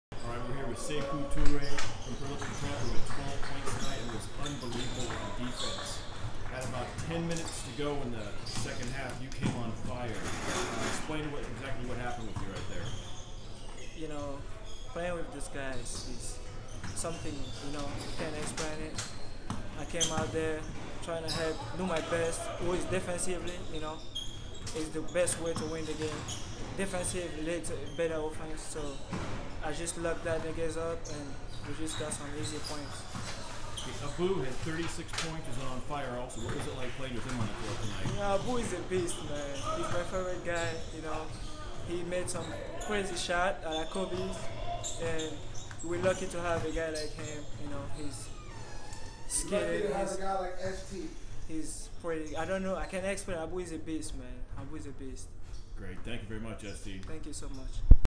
Prolific Preps Junior Toure
Junior Toure talks about the Crews victory over Aspire Academy yesterday in the Winter Shootout from Orlando Florida.